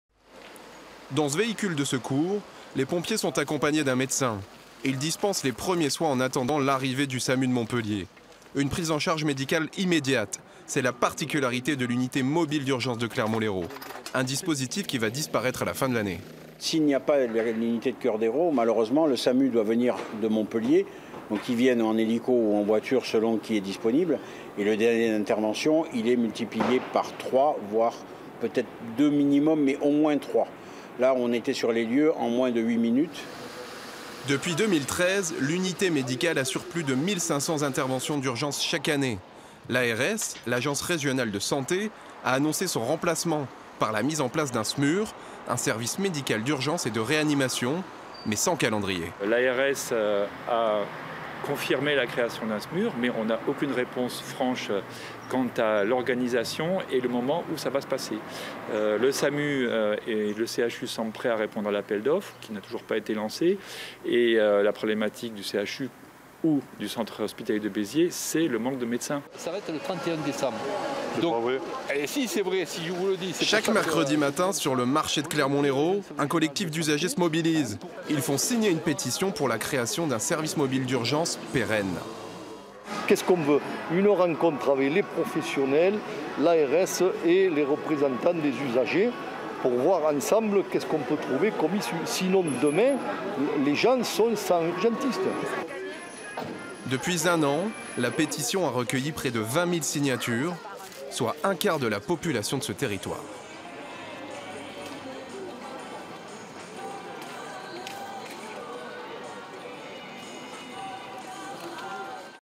Clermont-l'Hérault (34) : les riverains veulent sauver leur unité médicale d'urgence
Clermont-l'Hérault se mobilise pour sauver son unité médicale d'urgence. En expérimentation depuis 2013, afin de réduire le temps d'intervention des secours dans les communes proches du Salagou, cette structure devrait disparaître fin décembre. Une pétition est lancée pour obtenir son maintien.

 F3 LR - Reportage : C.Barbet et S.Djian